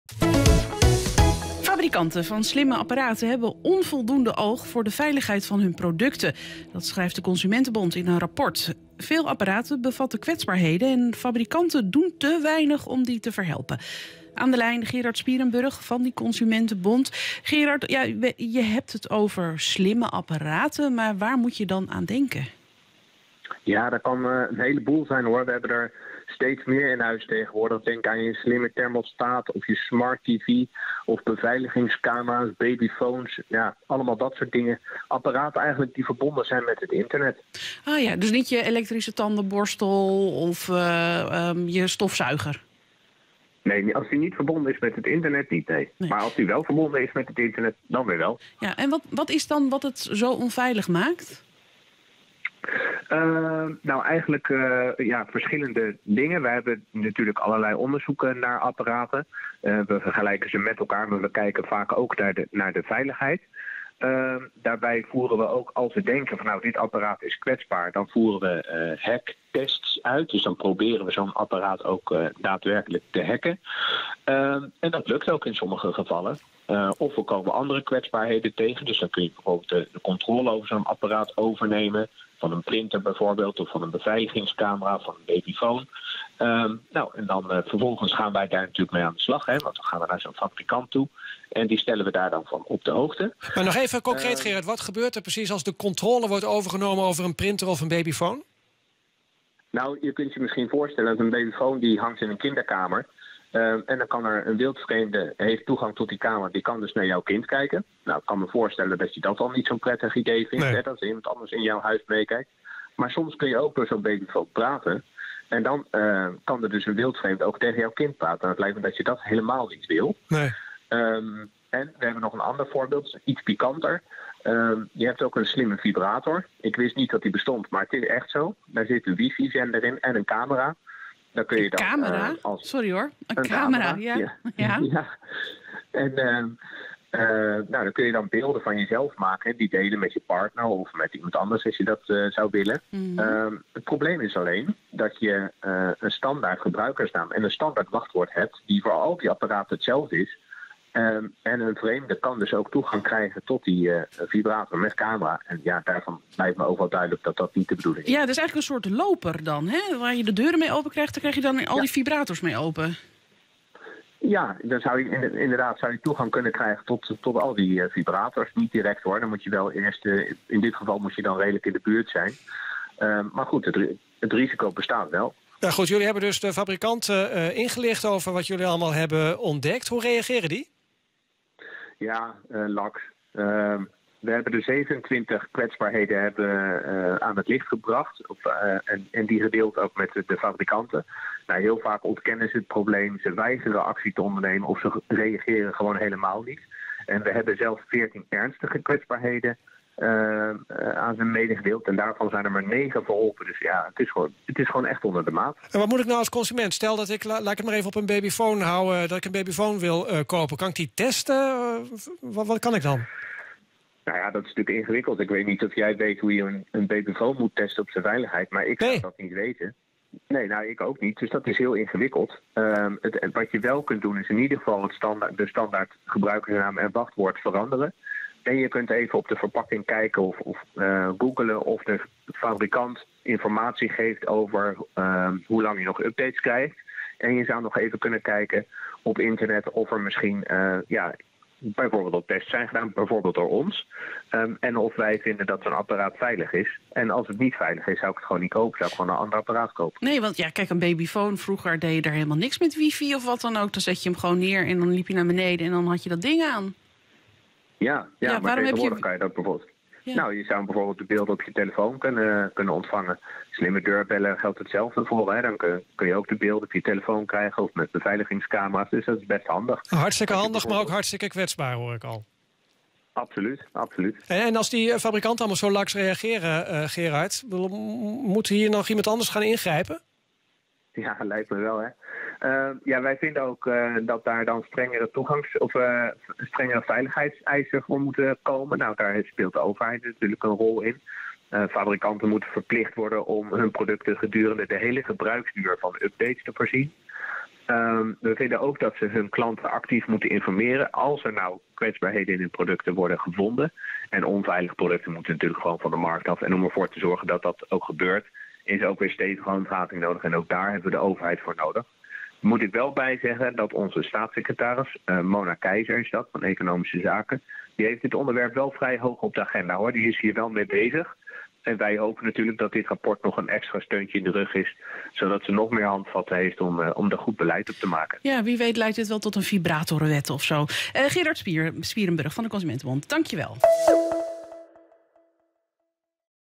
Consumentenbond: Hackers op de babyfoon
Fabrikanten van slimme apparaten hebben onvoldoende oog voor de veiligheid van hun producten. Dat schrijft de Consumentenbond in een rapport. Veel apparaten bevatten kwetsbaarheden en fabrikanten doen te weinig om die te verhelpen.

We belden erover met Gerard Spierenburg van de Consumentenbond.